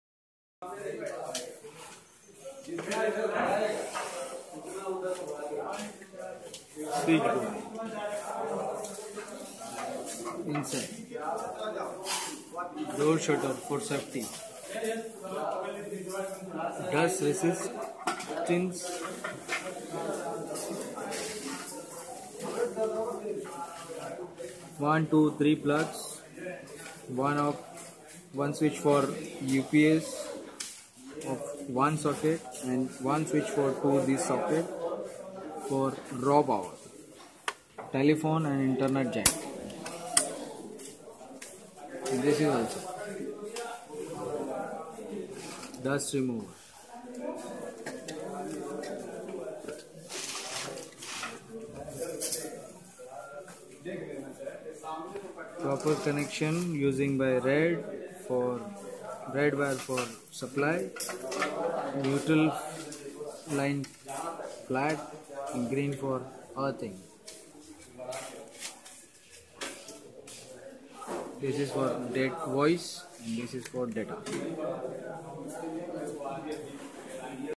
Popup Box
This is Popup Box.
This box is usually used in commercial and residential purpose.
This is 10 module metal body powder coated deep frame with openable shutter.
3 plugs are there.
1 for raw power.
Another 2 for UPS power.
The raw power socket will be operated by one switch which one you can see in the video.
As well as another 2 UPS power sockets operated by one switch.
One telephone socket and another one for data socket. Its called IO jack.
The blue color jack you can see behind is IO where CAT 5 or CAT 6 cable is terminated. its also called data jack RJ45.
Green one is telephone jack you can see behind is where CAT 5 cable is terminated. its called voice jack. Also called RJ11.

Connections:
The looping connections of UPS sockets and raw power sockets are shown in this video.
Here red color code is for live +ve black color for neutral and green color code is for earthing.
In this video, you can see the proper color code for connections and also assembling of switch board.